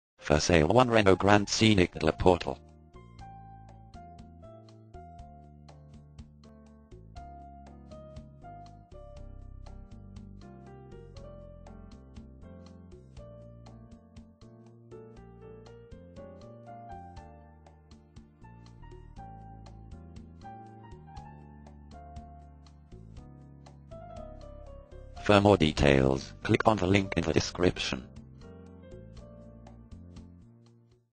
Sale one Renault Grand scenic Le Portel
For sale at  Le Portel  Used car Renault Grand scenic of 17000 Km.
used motors Autoenvideo : Sale one Renault Grand scenic  Le Portel  Pas-de-Calais